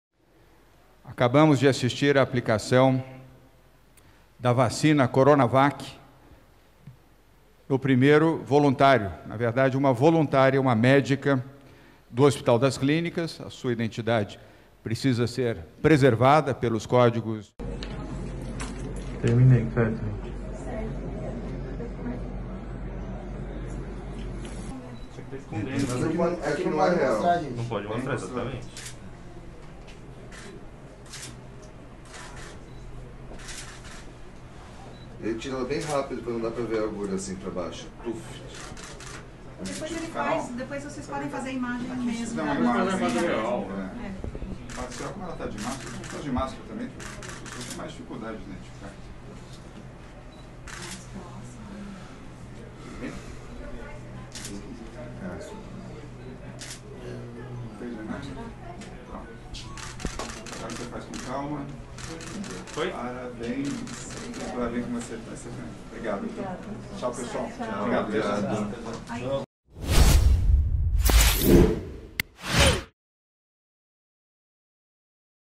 First volunteer receives COVID-19 vaccine in Brazil's Sao Paulo: governor
Sao Paulo Governor Joao Doria announces during a press conference that the first volunteer has received a dose of CoronaVac, a Chinese-made vaccine against COVID-19 now being tested in Brazil, one of the countries hit hardest by the pandemic, where deaths from the virus surge past 80,000.